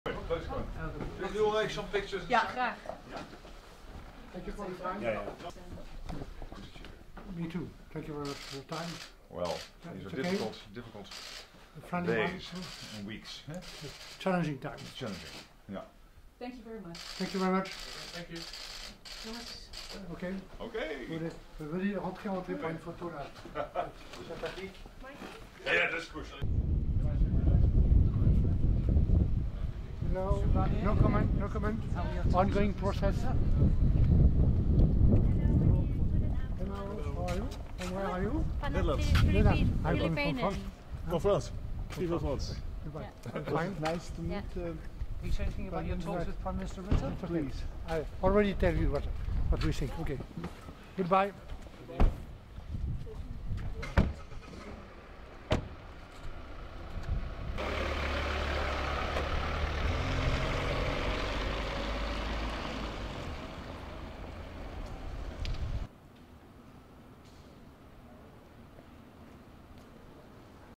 Barnier: we're experiencing 'challenging times'
(4 Feb 2019) The European chief Brexit negotiator Michel Barnier met with Dutch Prime Minister Mark Rutte in The Hague on Monday.
In a short press moment before the meeting, Rutte said these are difficult days and weeks.
Barnier agreed saying these are "challenging times".
Barnier arrived at the meeting accompanied by the most senior members of his team but left the meeting alone after 30 minutes.